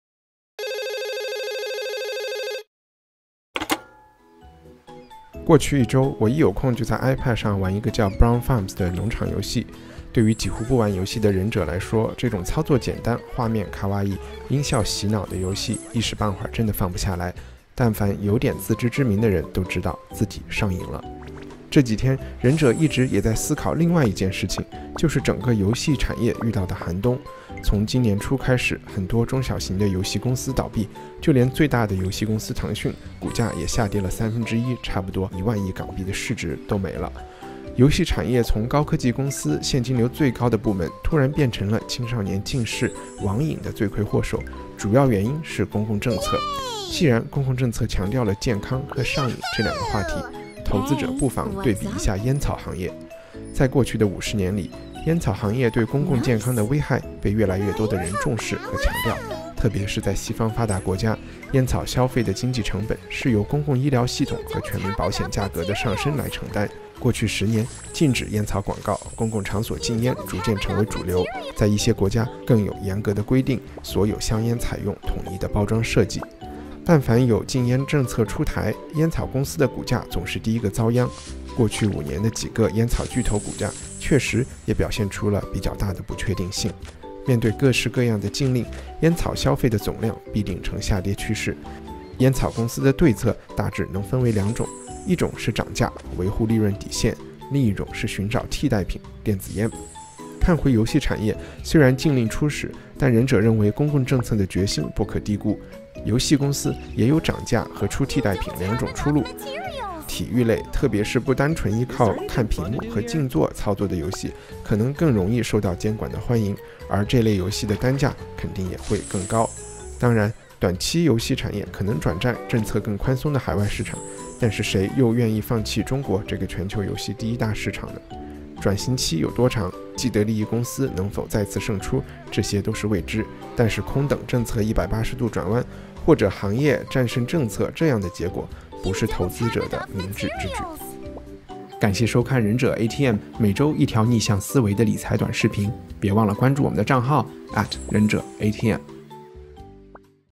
腾讯股票跌成狗，还能买吗？
游戏产业从高科技公司现金流最高的部门，突然变成青少年近视、网瘾的罪魁祸手，造成这个转变的主要原因是公共政策的转变。西方烟草行业的历史，其实给今天的中国游戏行业提供了最好的案例。